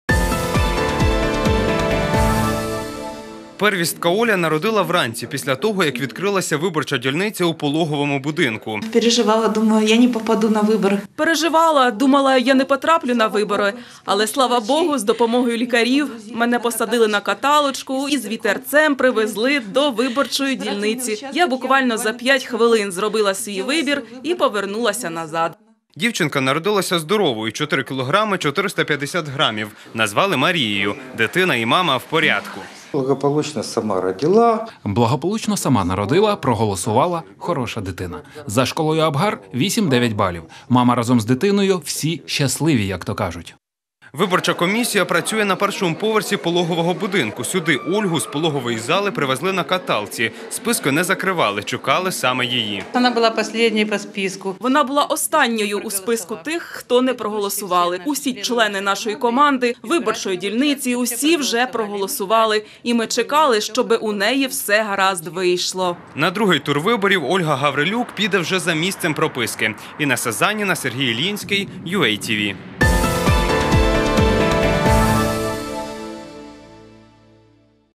Вибори-2019: киянка відразу після пологів вирушила голосувати
Народити й відразу ж проголосувати. Киянка Ольга Гаврилюк вирішила будь-що віддати свій голос за кандидата в президенти. До виборчої урни в пологовому будинку її привезли прямо з пологового залу. Зі щасливою мамою спілкувалася наша колега Інесса Заніна.